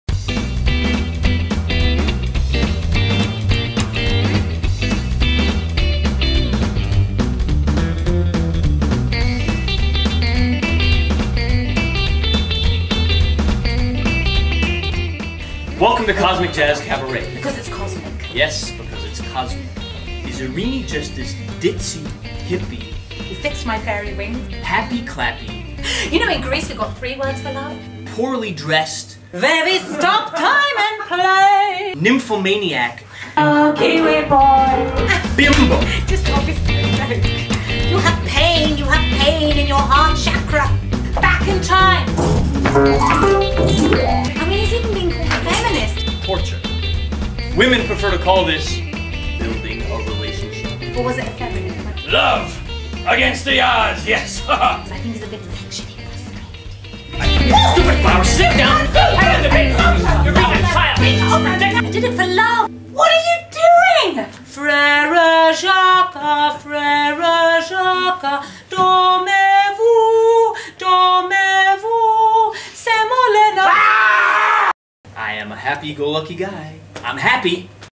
Cosmic Jazz Cabaret | Teaser
Ereni Mendrinos, flower-gazing cabaret-singer/comedienne/compulsive idealist and Robin Holloway, deadpan Chicago jazz-pianist and angst-ridden-philosophical-single-white-male present Cosmic Jazz Cabaret. Original songs, comedic banter, groovy jazz, great vocals, and philosophical ruminations about love and the meaning of life. Free Tea & Cake!